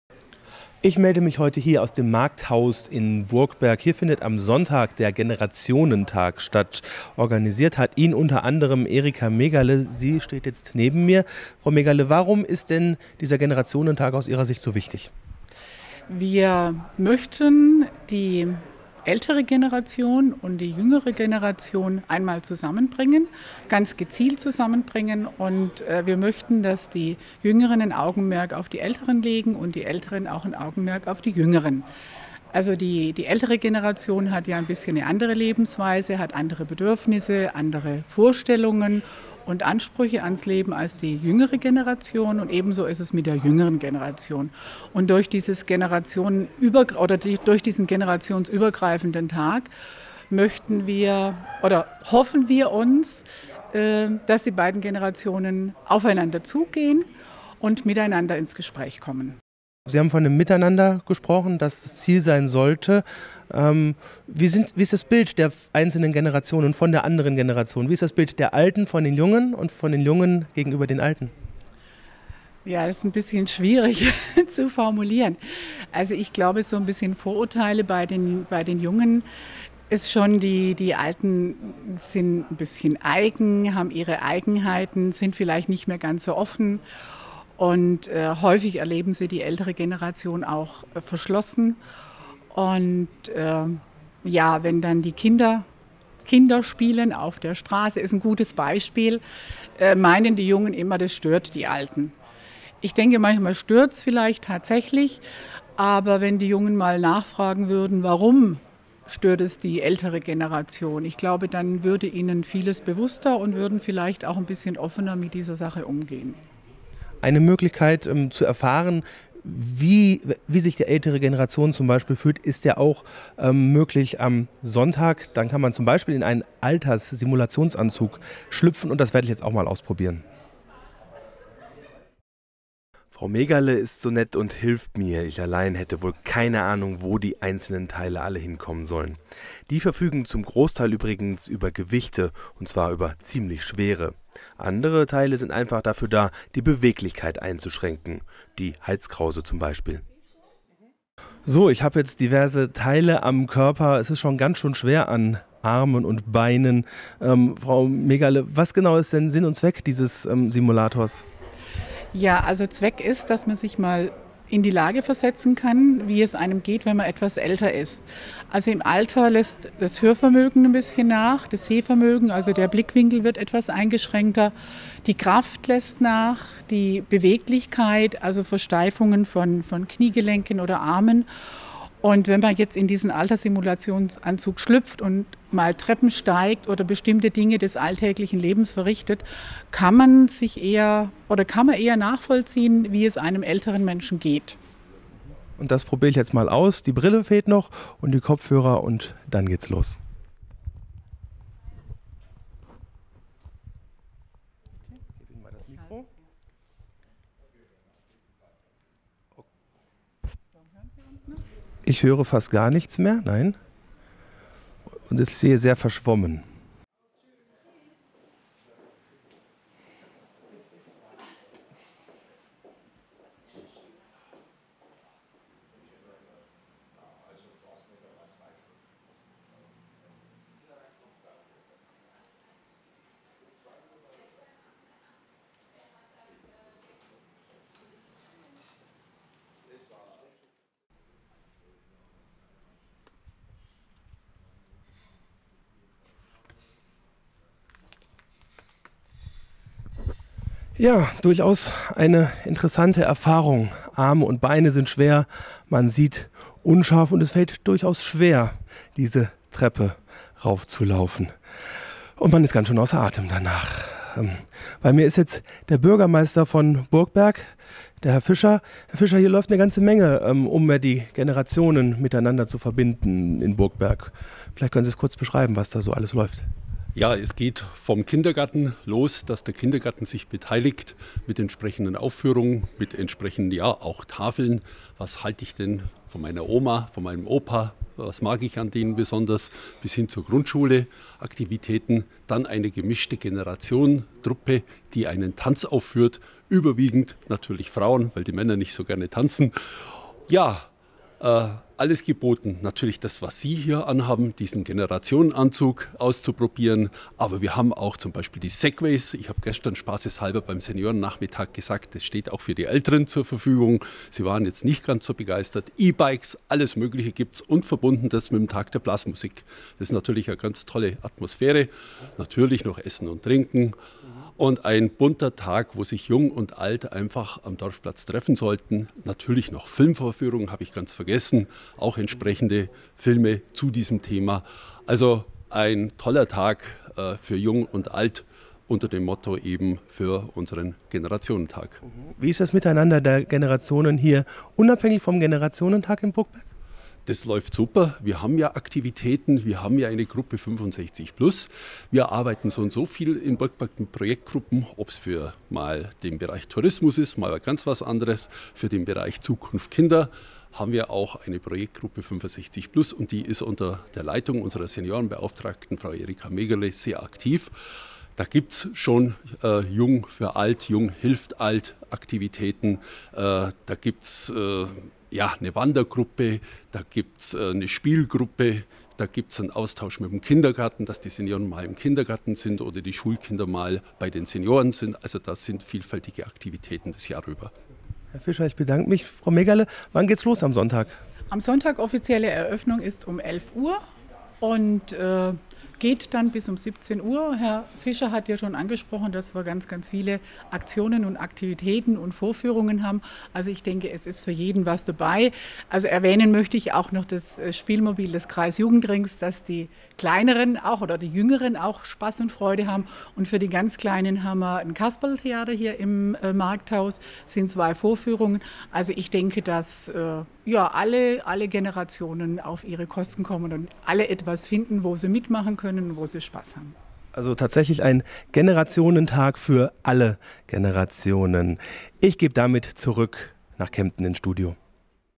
Mehr Verständnis fürs Alter - Der Selbsttest im Alters-Simulationsanzug
Drei oder vier Generationen unter einem Dach - Alt und jung, die sich gegenseitig helfen und unterstützen - in Zeiten, in denen Menschen wegen des Arbeitsplatzes hunderte Kilometer von der Heimat wegziehen müssen, nicht mehr überall alltäglich. Wo der tägliche Kontakt zwischen Alt und jung fehlt, da fehlt oft auch das Verständnis füreinander. Damit das in Burgberg nicht so ist, dafür tut die Gemeinde eine Menge. Am Sonntag findet zum Beispiel ein Generationentag statt, der beide Seiten zusammen führen soll.